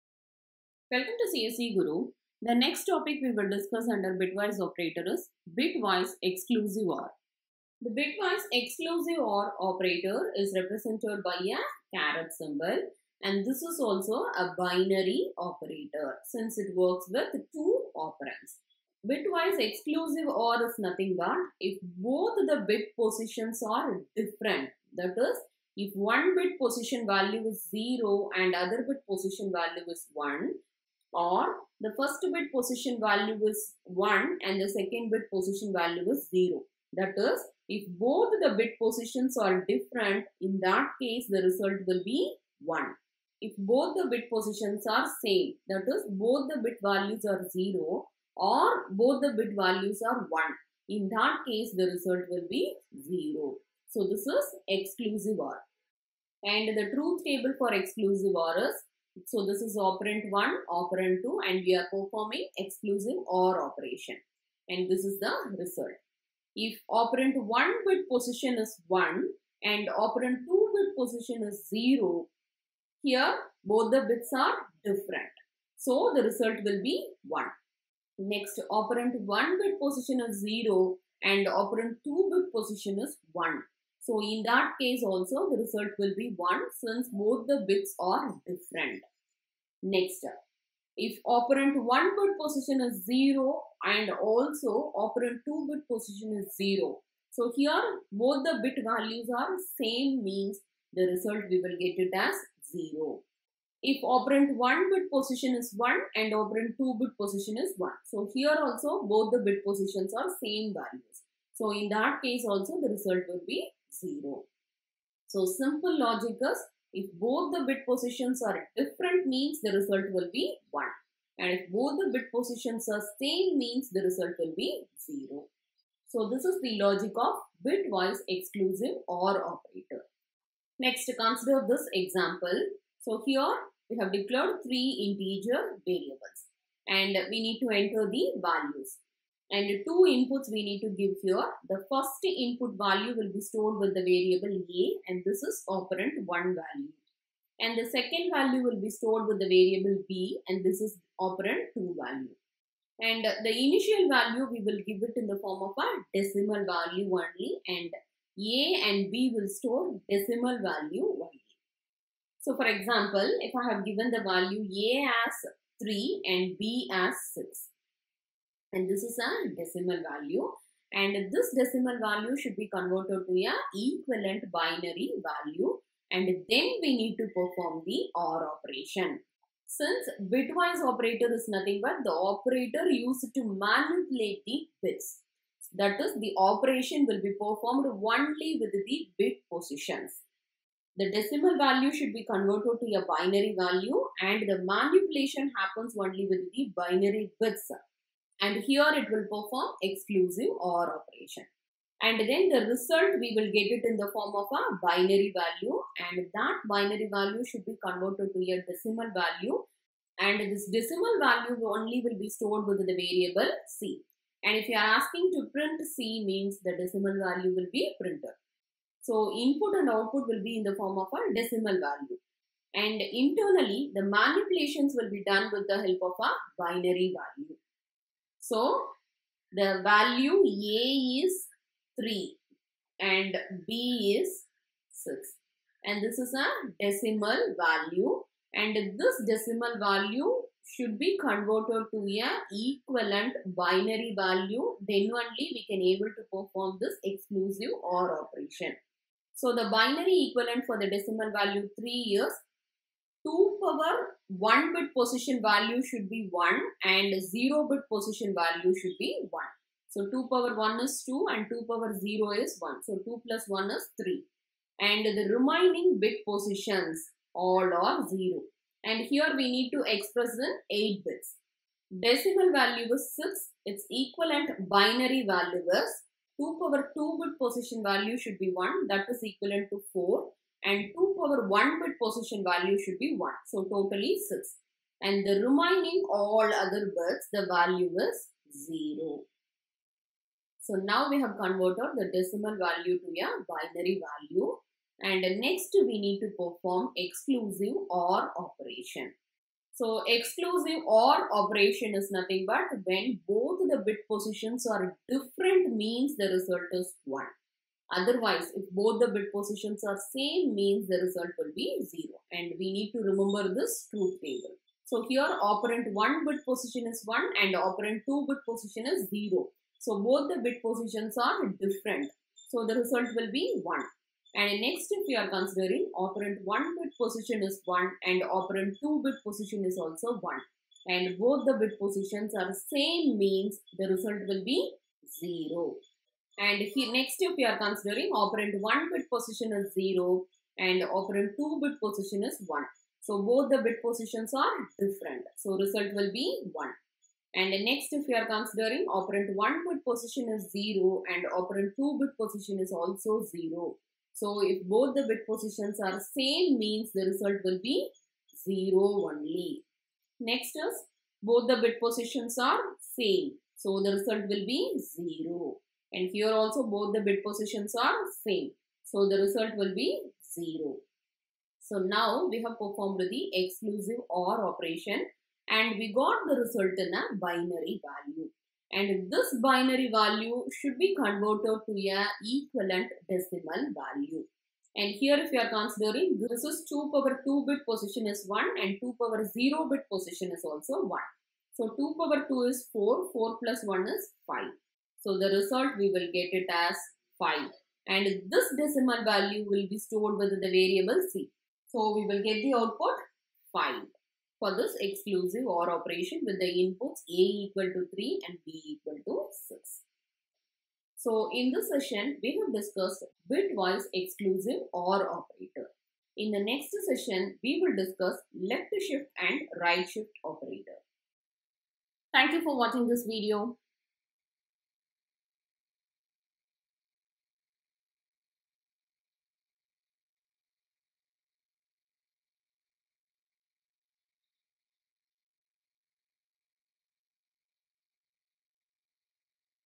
Bitwise XOR | Operators in C | Basic Concepts of C | Lec 26 | C Programming
C is the first and foremost programming language to programming. C is a high-level structured programming language developed by Dennis M Ritchie in the year 1972.

This video explains Operators in C Programming
Bitwise Operator
Operators used to manipulate the bits of given data

Bitwise XOR Operator
If  both the bit positions are different then result is1 otherwise 0

Truth Table
Op1      OR        op2         Result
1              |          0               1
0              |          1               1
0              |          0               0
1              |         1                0